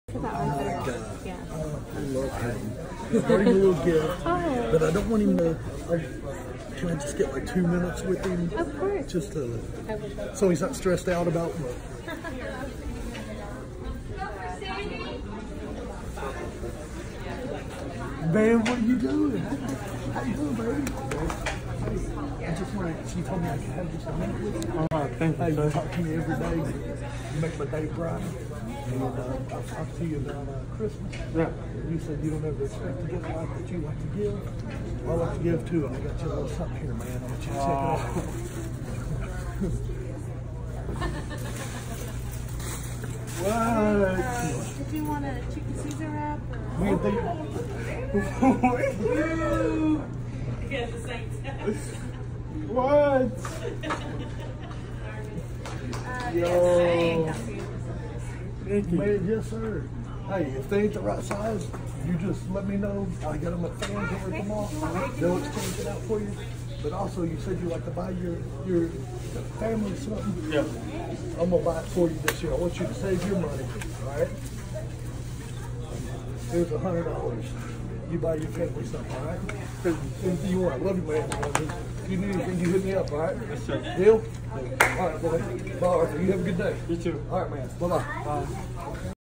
Christmas Spirit! McAlister’s Deli special worker.
My special friend Arthur at McAlister’s Deli is a blessing to all.